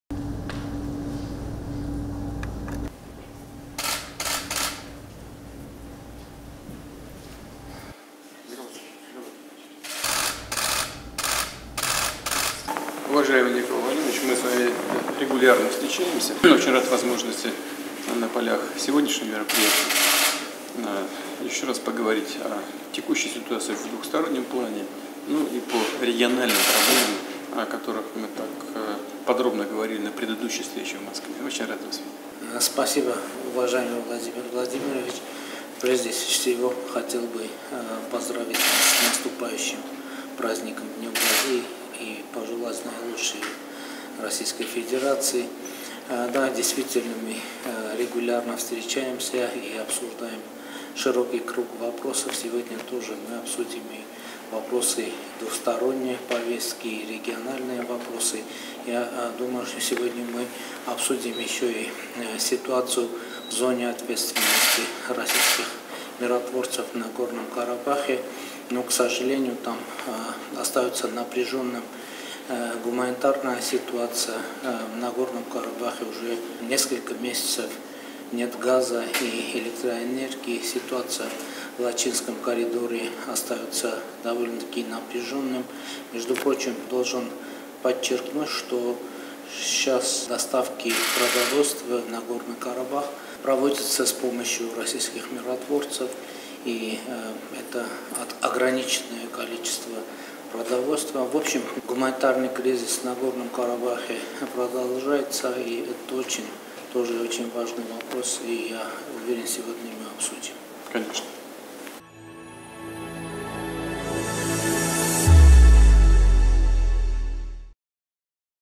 Հումանիտար ճգնաժամը ԼՂ ում շարունակվում է․ Փաշինյանը՝ Պուտինին
Վարչապետ Նիկոլ Փաշինյանը Սոչիում հանդիպում է ունեցել Ռուսաստանի Դաշնության նախագահ Վլադիմիր Պուտինի հետ:

ՌԴ նախագահ Վլադիմիր Պուտին - «Հարգելի Նիկոլ Վլադիմիրի, մենք պարբերաբար հանդիպում ենք, շատ ուրախ եմ այսօրվա միջոցառումների շրջանակում ևս մեկ անգամ խոսել երկկողմ հարաբերությունների ընթացիկ իրավիճակի և տարածաշրջանային հարցերի շուրջ, որոնց մասին մենք մանրամասն խոսել ենք նախորդ հանդիպման ընթացքում: Շատ ուրախ եմ տեսնել Ձեզ»:

Վարչապետ Նիկոլ Փաշինյան - «Շնորհակալություն հարգելի Վլադիմիր Վլադիմիրի, առաջին հերթին ցանկանում եմ շնորհավորել Ձեզ գալիք Ռուսաստանի օրվա առիթով և մաղթել լավագույնը Ռուսաստանի Դաշնությանը: Այո, իսկապես, մենք պարբերաբար հանդիպում ենք և քննարկում հարցերի լայն շրջանակ: Այսօր ևս կքննարկենք երկկողմ օրակարգի և տարածաշրջանային հարցեր: Կքննարկենք նաև ռուսական խաղաղապահների պատասխանատվության գոտում` Լեռնային Ղարաբաղում իրավիճակը, ցավոք այնտեղ հումանիտար իրավիճակը շարունակում է լարված մնալ: Լեռնային Ղարաբաղում արդեն մի քանի ամիս չկա գազ և էլեկտրաէներգիա, Լաչինի միջանցքում իրավիճակը շարունակում է մնալ բավականին լարված: Իմիջիայլոց, պետք է ընդգծեմ, որ այժմ մթերքների մատակարարումները Լեռնային Ղարաբաղ իրականացվում է ռուսական խաղաղապահների աջակցությամբ, դա սահմանափակ քանակի մթերքներ են: Հումանիտար ճգնաժամը Լեռնային Ղարաբաղում շարունակվում է և դա նույնպես շատ կարևոր հարց է ու վստահ եմ այսօր կքննարկենք»:

Տեղի է ունեցել նաև Եվրասիական միջկառավարական խորհրդի և ԱՊՀ կառավարությունների ղեկավարների խորհրդի նիստերի մասնակիցների հանդիպումը ՌԴ նախագահի հետ: